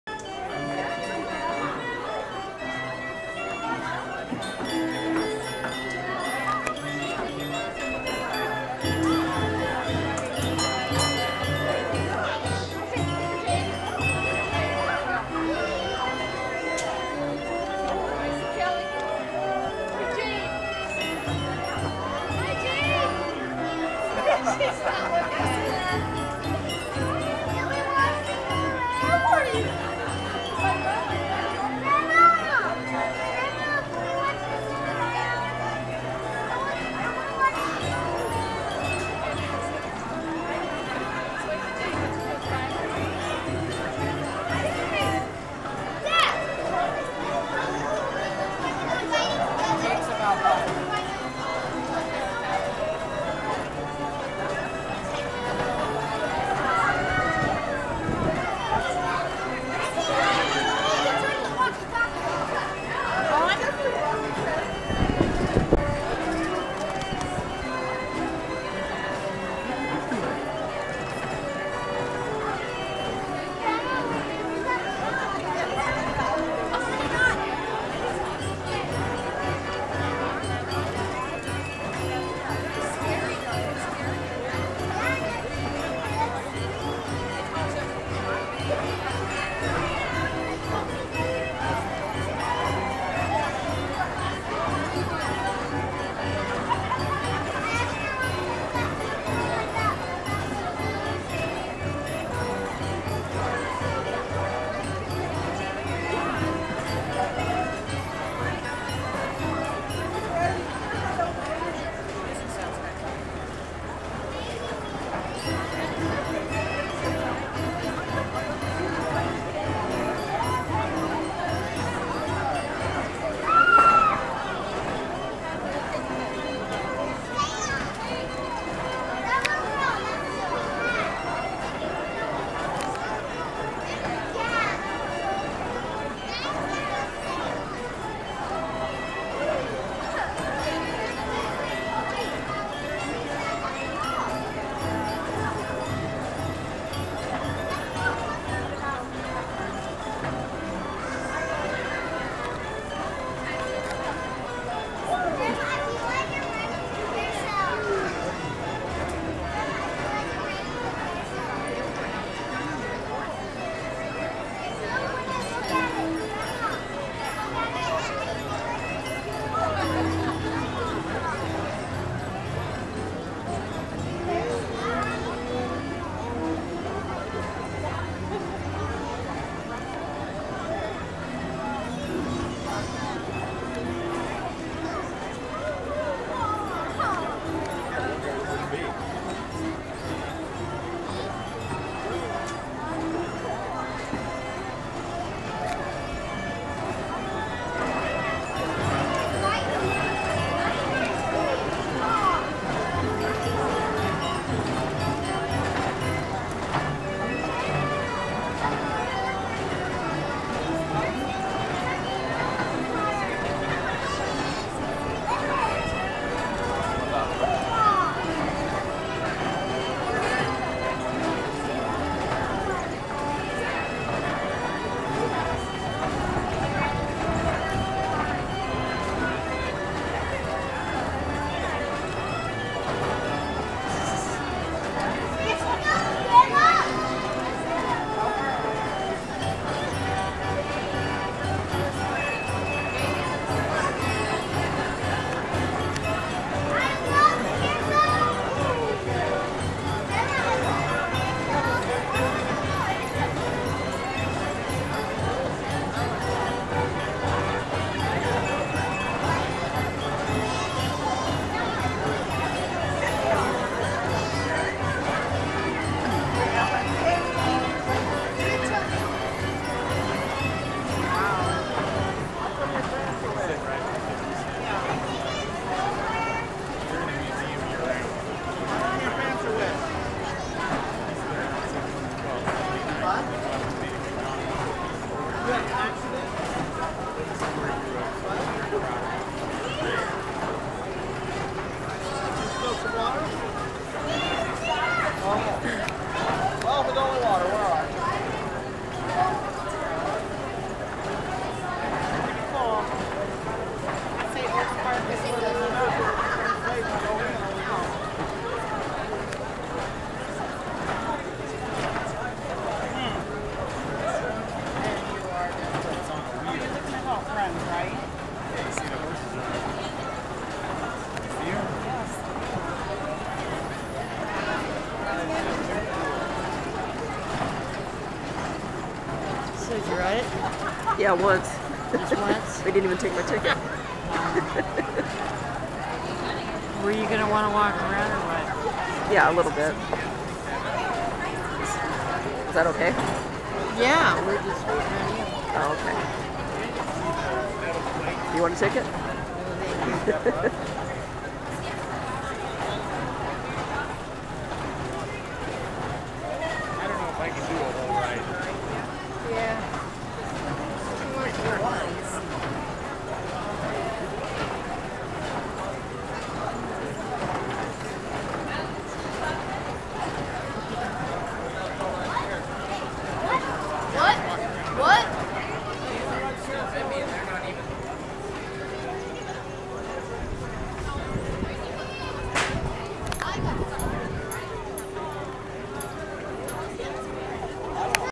Bickleton Carousel (Off Ride)
Very rare, primitive Herschell-Spillman track-style "riding gallery" carousel at the rodeo near Bickleton, WA.  This type of carousel, instead of the horses being mounted on poles and going gently up and down, are fixed onto a rocking mechanism that automatically "gallops" them as the ride spins, producing a wild ride more like that of a real horse.

There were virtually no videos of this carousel posted online that I could find, so I thought it needed some documentation.